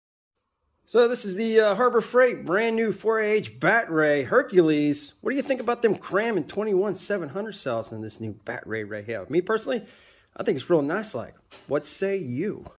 New Harbor Freight Extreme Battery
This is the new Harbor Freight Extreme Performance 4ah battery with 21700 cells